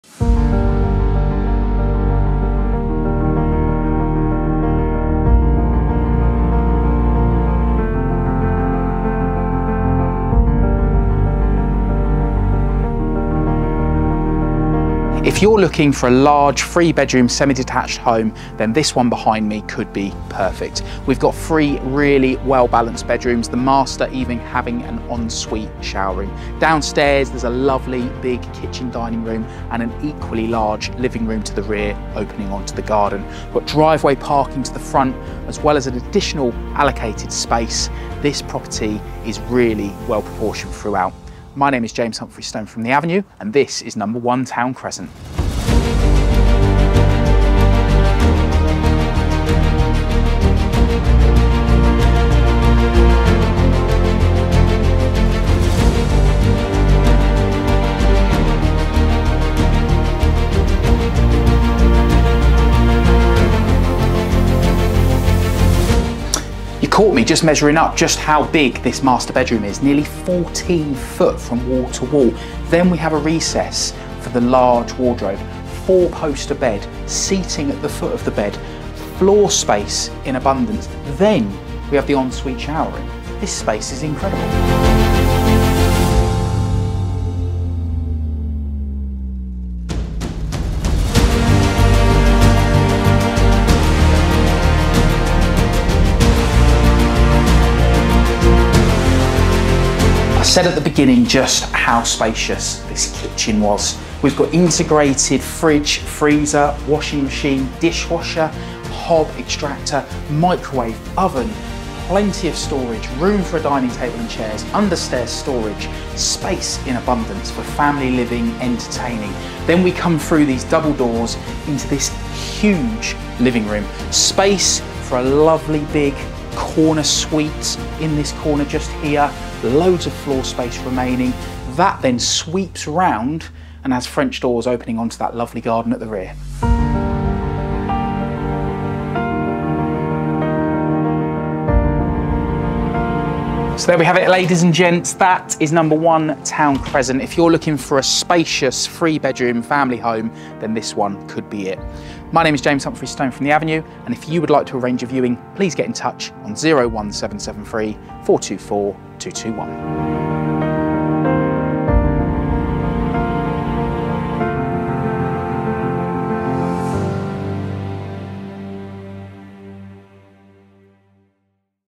Deceptively Large Three Bedroom Home In Pinxton | Nottinghamshire
Welcome to this spacious 3-bedroom house located on Town Crescent, Pinxton. With its appealing features, including a luxurious en-suite master bedroom, a large kitchen/dining area, and a beautiful garden, this property is perfect for families and individuals alike.

As you enter the house, you are greeted by a large and inviting entrance hall. The hallway gives access to a downstairs cloakroom but also leads you to the large kitchen/dining area. The kitchen is equipped with a range of integrated appliances, making cooking and entertaining effortless. The ample space allows for a dining table, perfect for family meals or hosting gatherings with friends.

Double doors open to a spacious living room, which boasts plenty of natural light streaming through the large windows, creating a bright and airy ambiance. The living room also features French doors that open up to the garden, allowing for seamless indoor-outdoor living during the warm summer months.

Upstairs, you will find three generously sized bedrooms, offering plenty of space for relaxation and rest. The highlight of this property is undoubtedly the en-suite master bedroom. This private retreat is the epitome of luxury, providing a tranquil space to unwind after a long day. The en-suite bathroom features modern fixtures and fittings, including a stylish shower.

Additionally, the house includes a main bathroom, perfect for guests or family members.

Outside, the garden is easily maintained, allowing for leisurely outdoor activities, barbecues, or simply enjoying the fresh air. The garden is an extension of the house, inviting you to create lasting memories with your loved ones.

Town Crescent, Pinxton offers an idyllic neighbourhood to call home. With its close proximity to local amenities, schools, and transport links, you'll find everything you need within easy reach.